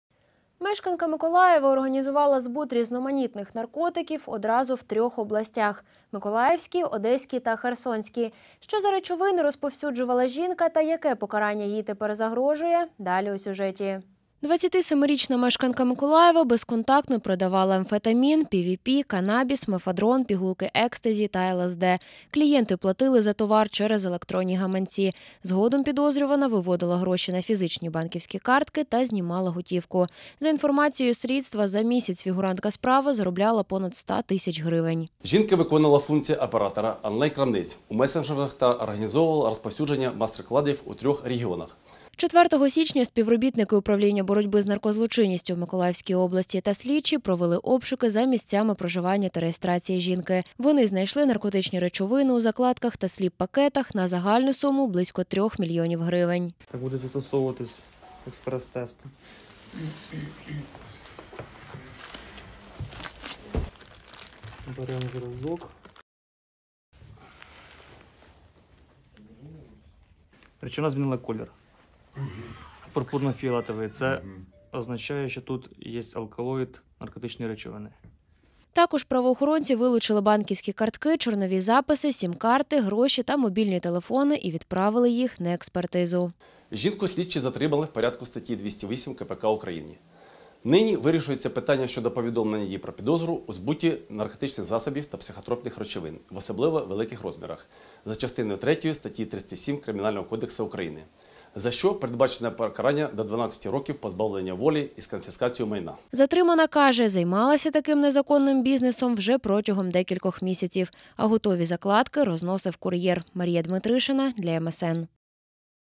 Канабіс, амфетамін, екстезі: у Миколаєві затримали жінку, яка продавала наркотики у трьох областях
Мешканка Миколаєва організувала збут різноманітних наркотиків одразу в трьох областях –Миколаївській, Одеській та Херсонській.